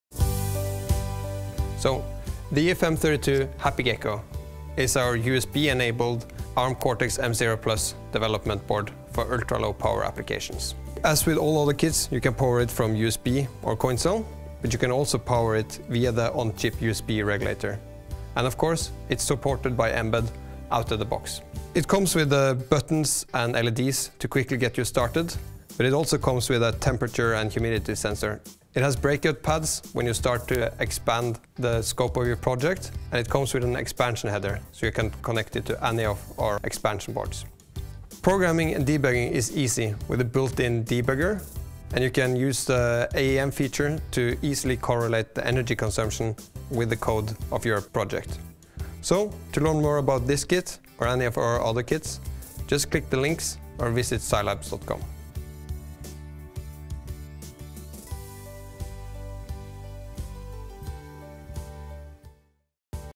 EFM32 Happy Gecko Cortex-M0+ USB Starter Kit from Silicon Labs
Silicon Labs' low energy SLSTK3400A provides an excellent tool to get familiar with the EFM32 USB-enabled Happy Gecko 32-bit microcontrollers (MCUs).

Our EFM32 product manager Alf Petter Mossevig gives an overview of the EFM32 Starter Kits, the industry’s most energy-friendly MCU based on the ARM® Cortex®-M0+ processor.

Supported by Simplicity Studio, the starter kit contains sensors and peripherals, touch-buttons, LEDs, Si7021 Digital Relative Humidity and Temperature Sensor, memoryLCD and simplicity header, demonstrating some of the MCU's many capabilities and can serve as a starting point for application development. The SLSTK3400A includes AEM (Advanced Energy Monitoring) and the Energy Profiler capabilities allowing real-time energy profiling of your application. This kit has two AEM-channels, one for measuring the current consumption in self-powered mode, and one for monitoring the VBUS-current when the chip is USB-powered.